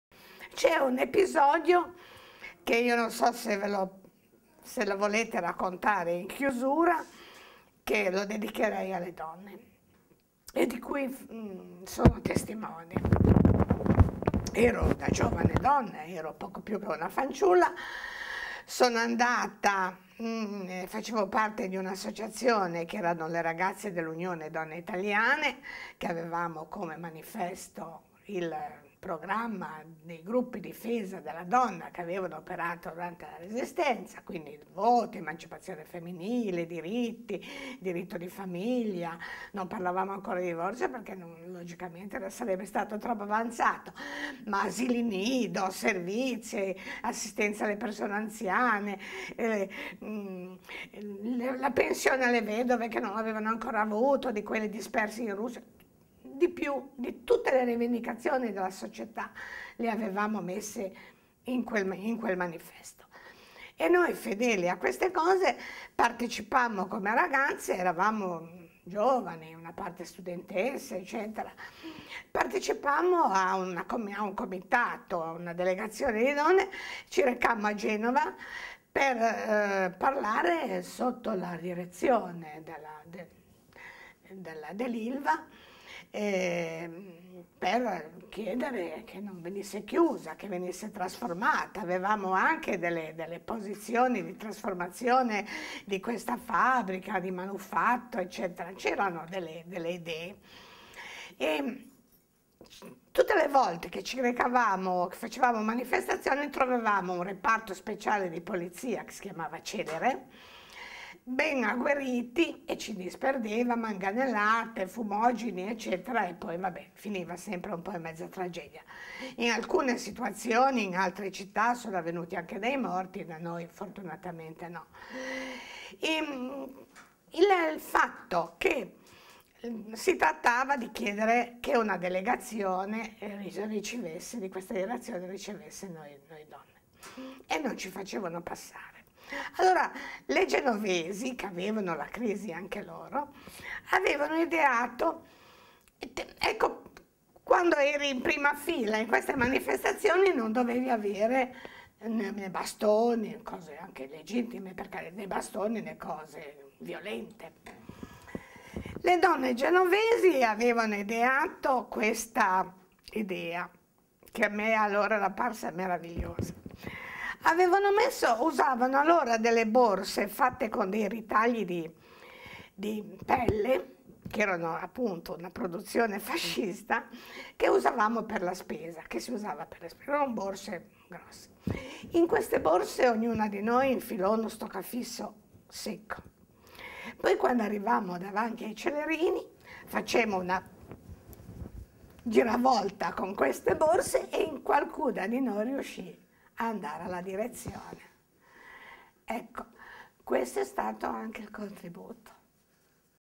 LA FABBRICA E LA CITTÀ - BOLLA / LE LOTTE DELLE DONNE NEGLI ANNI '50
Intervista a Maria Bolla pubblicata il 22 gennaio 2013.

Presidente dell'ANED (Associazione Nazionale Deportati Politici) racconta un episodio relativo alle lotte delle donne dell'ILVA negli anni '50... quando per difendersi dalle cariche della celere si utilizzava lo stoccafisso!

LABORATORIO AUDIOVISIVI BUSTER KEATON
CAMPUS UNIVERSITARIO DI SAVONA